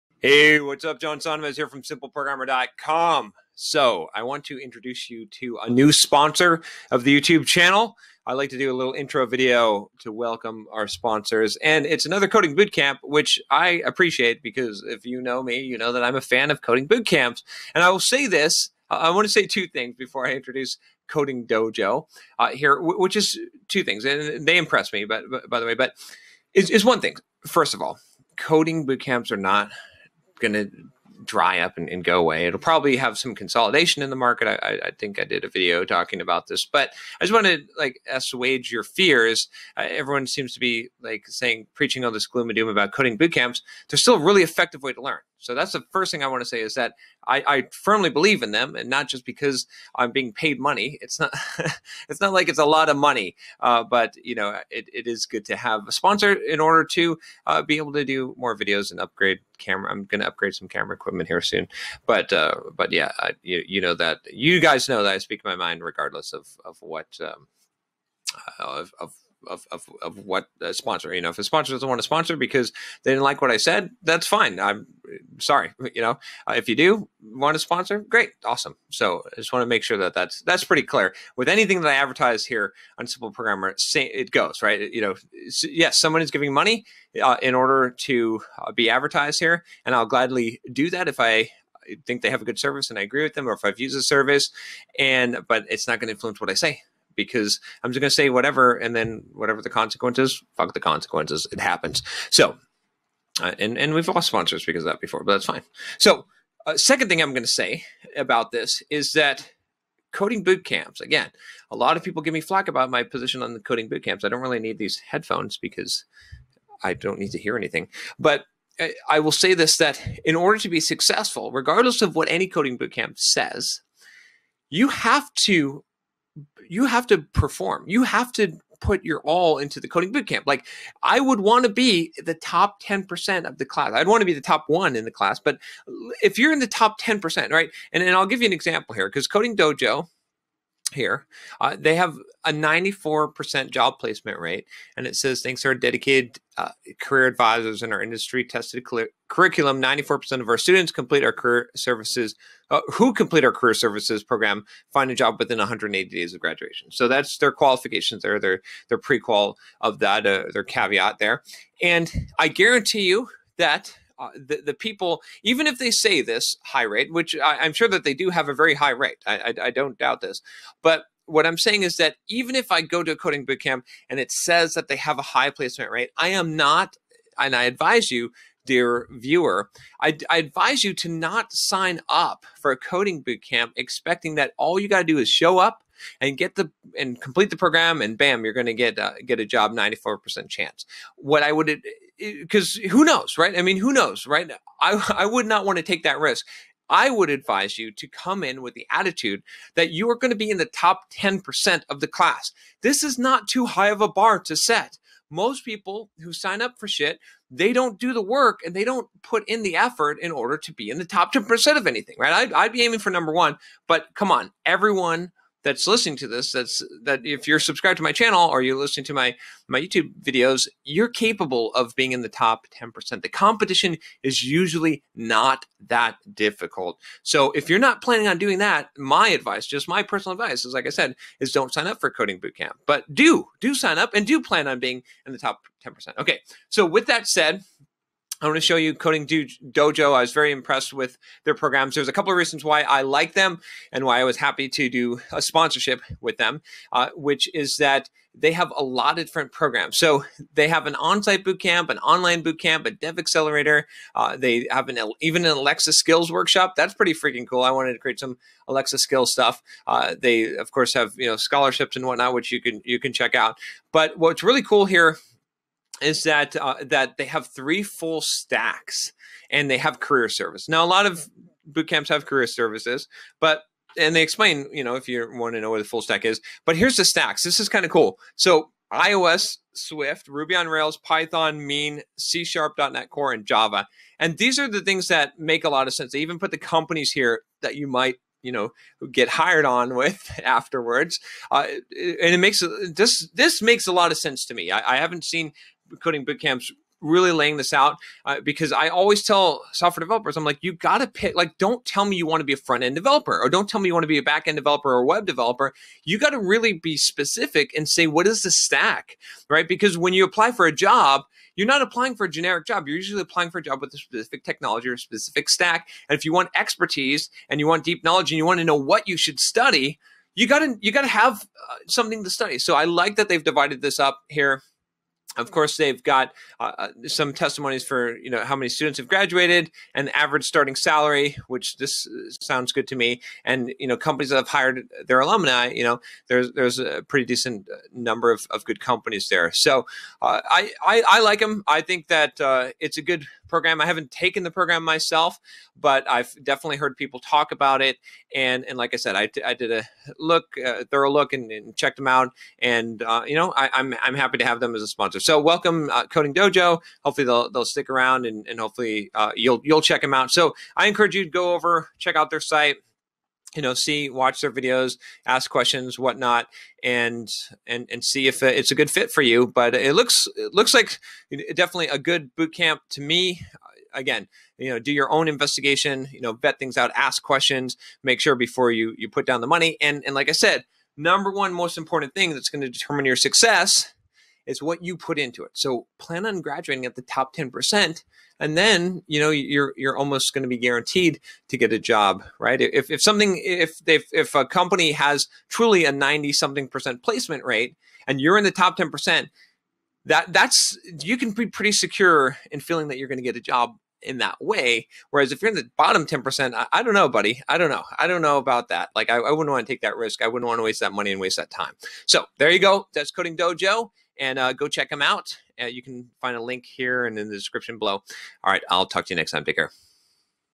Coding Dojo: Teach You Programming In 14 Weeks?
Simple Programmer

Today I'd like to introduce you to Coding Dogo, our new sponsor.

Coding Dojo is a coding Bootcamp that aims to teach you programming in just... 14 weeks? Yeah, you heard me right.

Besides that, they have an amazing education structure, which presents us with "stacks", which are the things you need to pick in order to choose what to learn.

So, is Coding Dojo a good option when it comes to Coding Bootcamps?